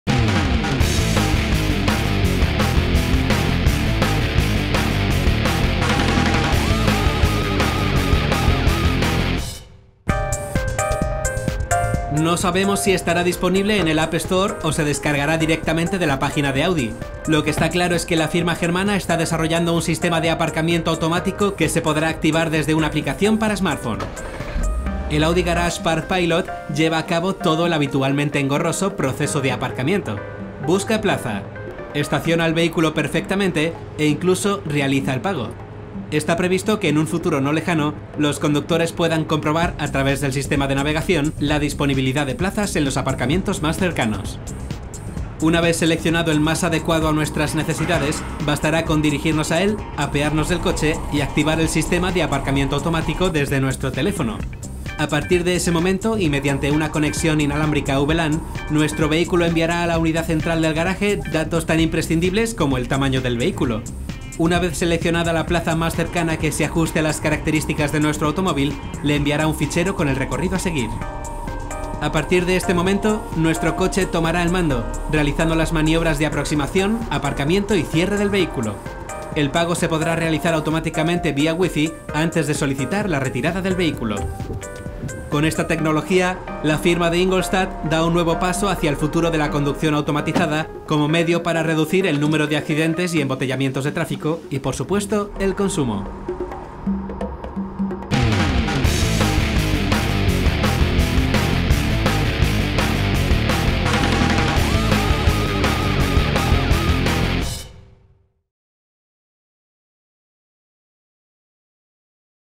Aparcamiento Pilotado de Audi
No se si estará disponible en el App Store o si se descargará directamente de la página de Audi. Lo que está claro es que la firma germana está desarrollando un sistema de aparcamiento automático que se podrá activar desde una aplicación para Smartphone.